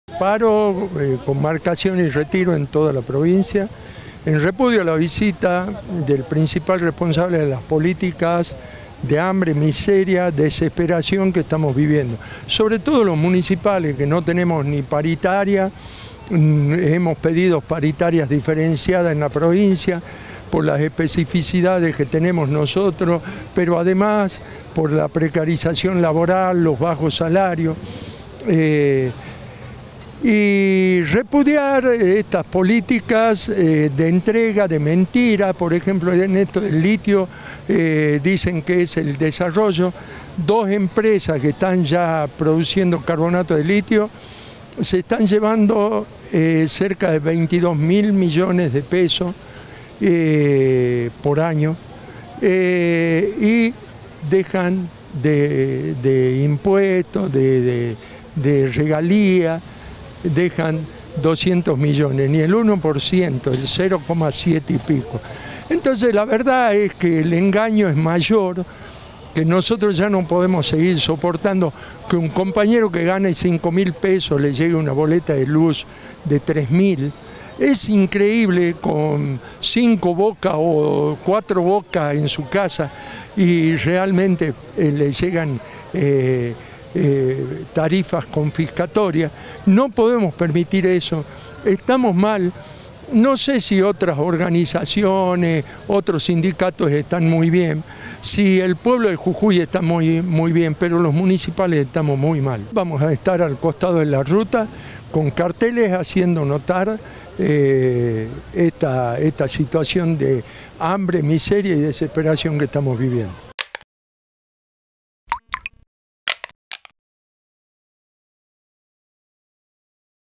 Paro y movilización en las rutas de la provincia
Seom, Barrios de Pie, Cedems y FOL realizan distintas medidas de fuerza ante la llegada del mandatario.